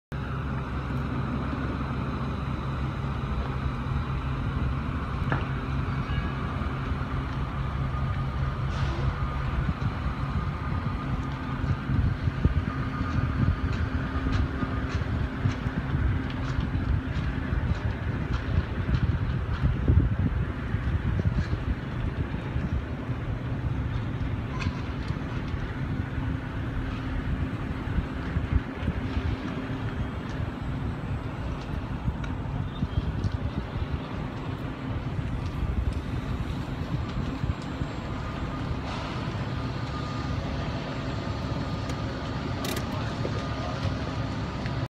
Fire
O'Reilly auto parts burns down